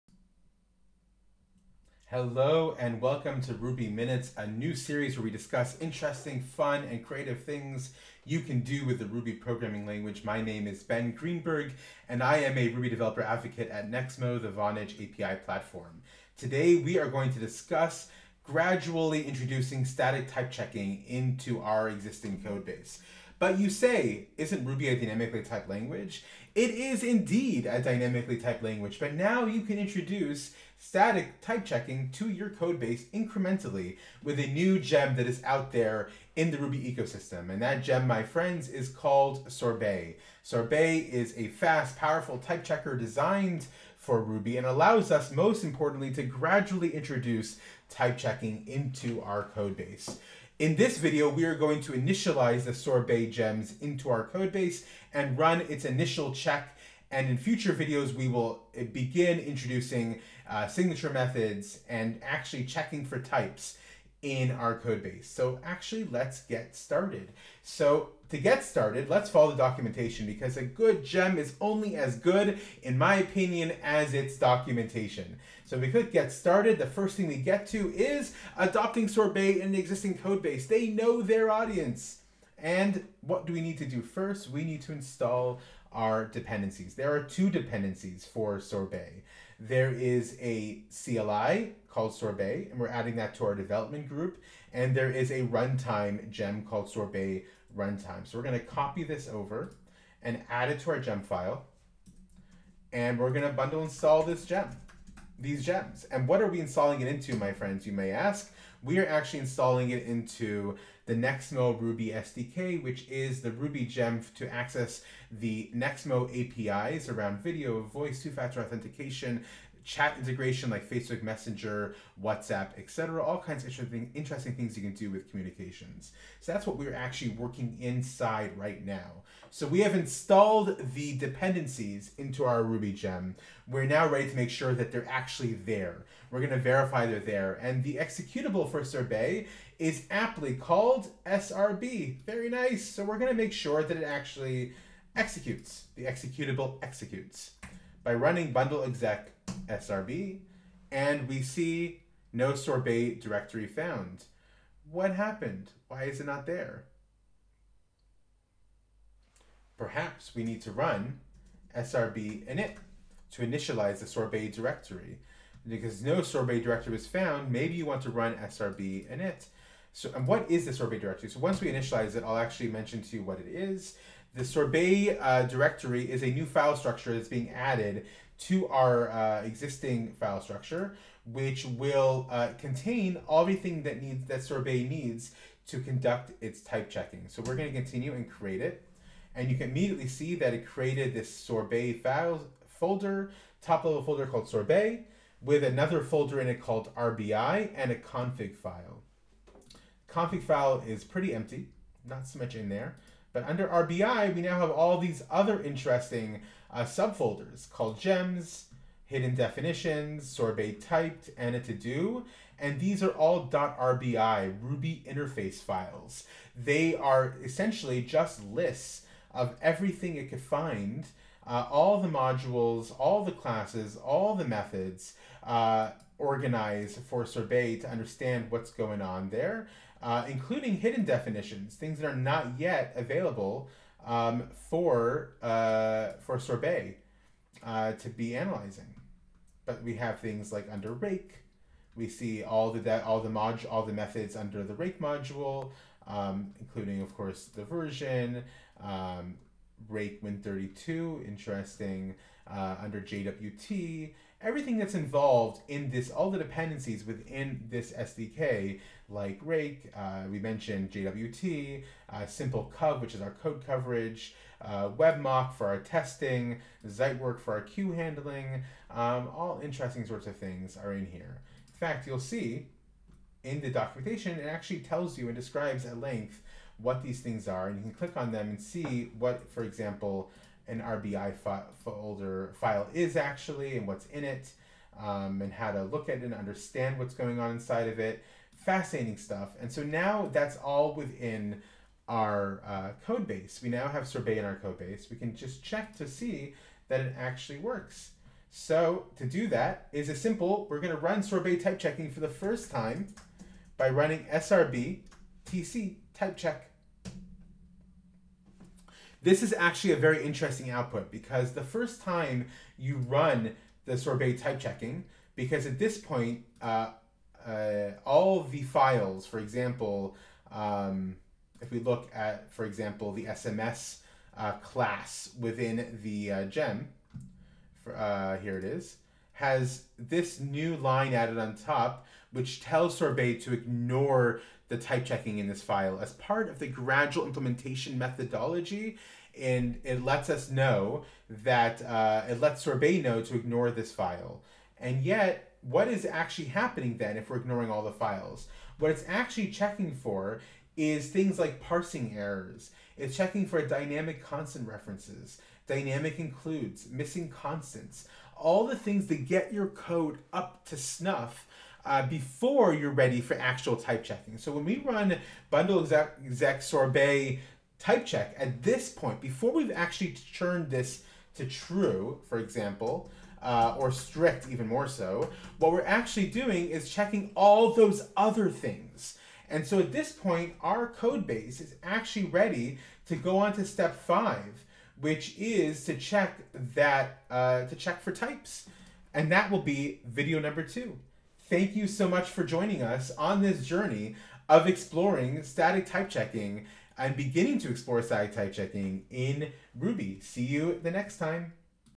Static Type Checking in Ruby Part 1 | Vonage Gems
In this video, we introduce the topic of static type checking in Ruby and begin working with Sorbet, the new static type checking Ruby gem. We install it in our application and perform the first checks with it.
Ruby is a traditionally dynamically typed language, and for those of us who love Ruby, we love the flexibility that comes with that! However, there are new winds blowing in the Ruby world, and they are introducing static typing to Ruby. There are many benefits to static type checking in a codebase. The primary benefit is improved developer experience for those who integrate with the applications we write. Static type checking can produce more confidence for those developers in our classes, methods, and variable definitions.
In this 3-part series, we will explore the beginnings of getting up and running with static type checking in Ruby using Sorbet, a new static type checking gem. We will show how to integrate it into your application, perform the first code quality checks and build our first method signatures.